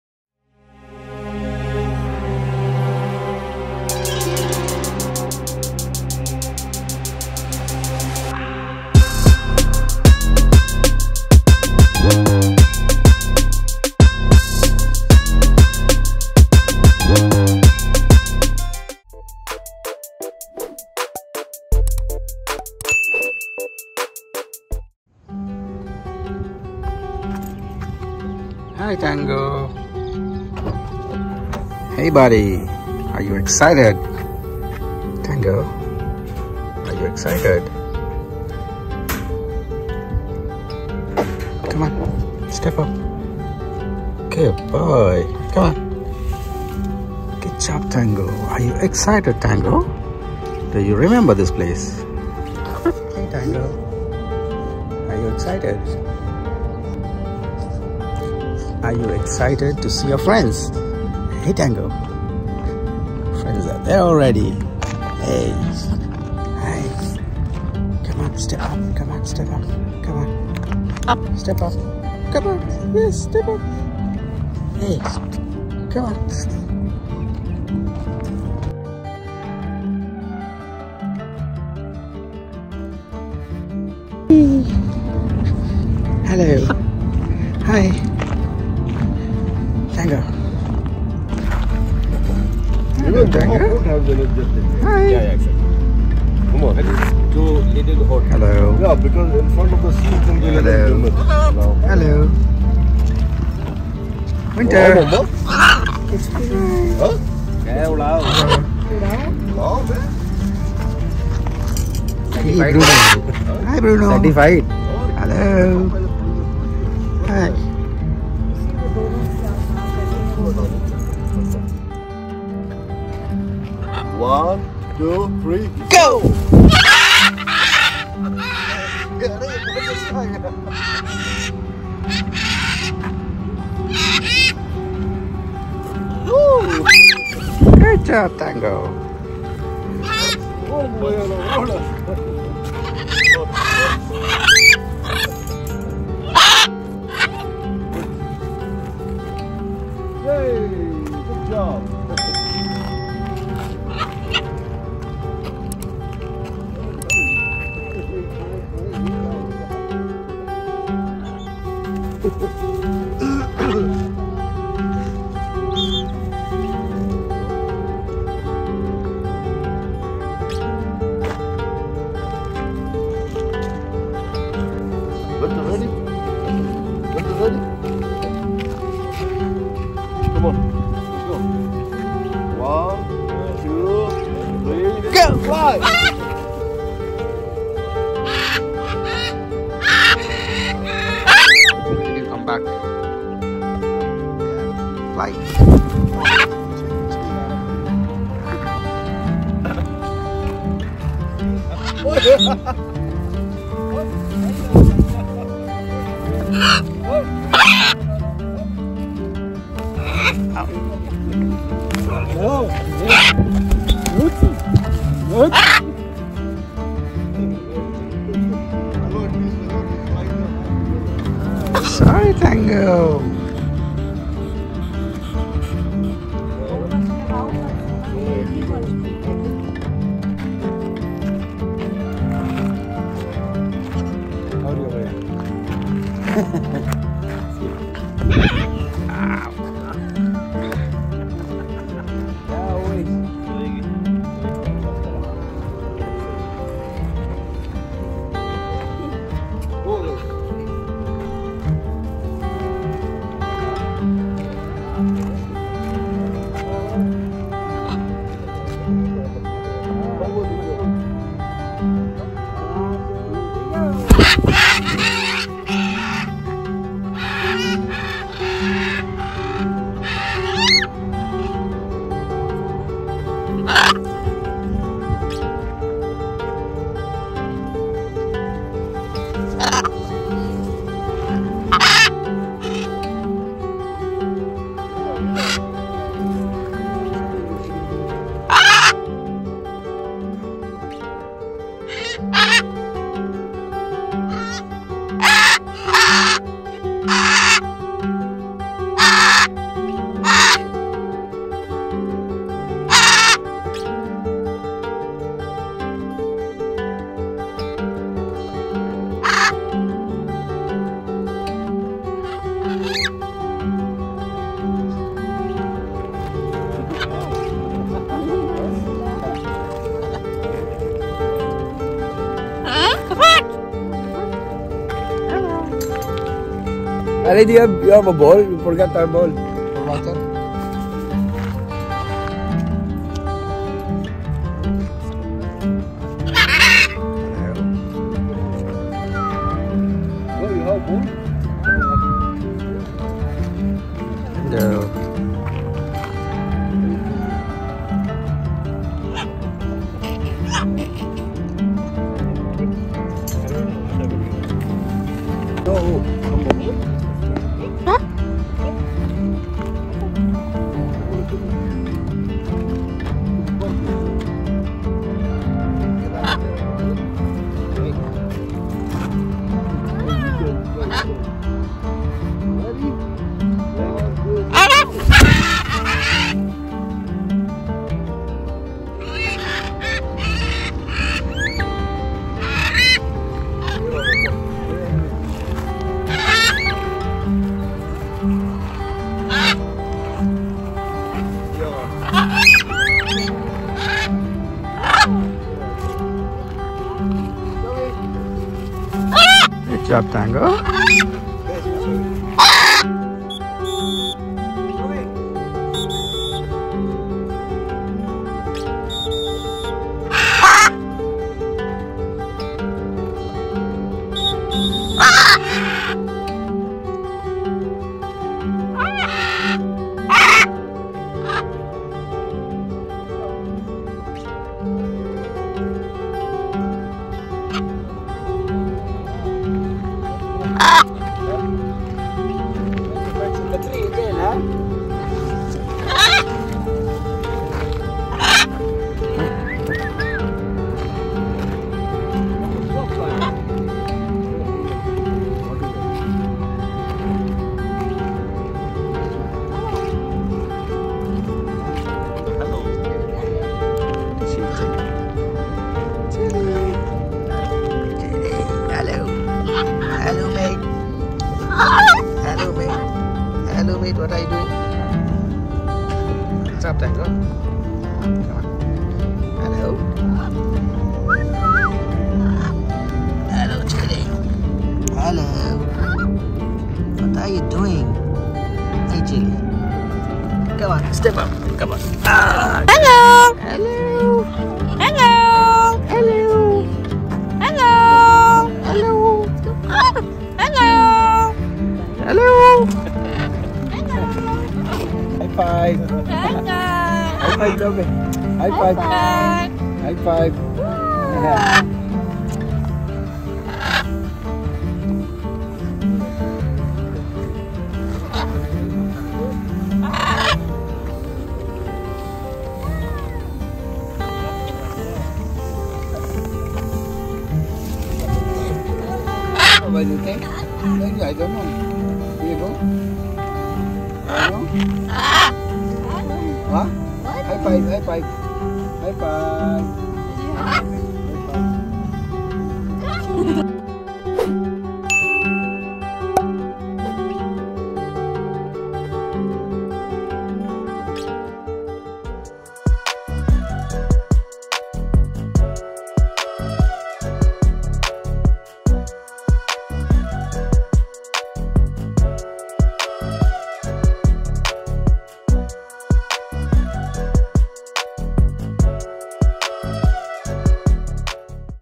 Amazing Free-Flight Parrots | Wings of Freedom | Rifshy RnS | 23-Sept-2023
Wings of Freedom “Welcome to the Magnificent World of Free-Flight”

Prepare to be captivated by the breathtaking world of free-flight parrots!
Join us on an incredible journey through lush, sun-kissed landscapes as these vibrant birds spread their wings and take flight.

In this video, you'll witness the stunning beauty and remarkable adaptability of free-flying parrots as they explore their urban habitats. These feathered acrobats have made these unexpected places their home.

Join us on this journey of wonder and admiration for the resilience and adaptability of free-flight parrots.

🔔 RingTheBell so you will not miss my upcoming videos